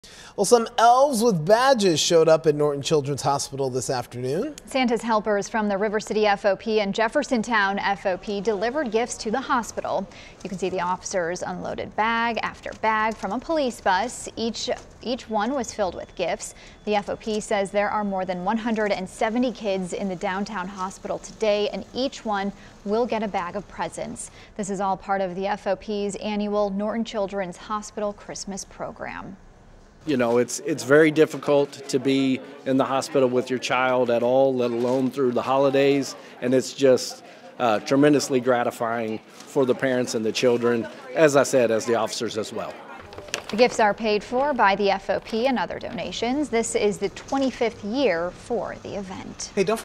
Louisville police officers deliver bags of gifts to Norton Children's Hospital
Members of local Fraternal Order of Police organizations surprised patients at Norton Children's Hospital with gifts on Monday afternoon.

River City FOP Lodge No. 614 and Jeffersontown FOP Lodge No. 26 delivered gifts to children at the hospital in downtown Louisville. Officers unloaded bags of toys from a police bus.

The FOP said there are more than 170 kids in the hospital, and each one will get a bag of presents. It's part of the FOP's annual Norton Children's Hospital Christmas program.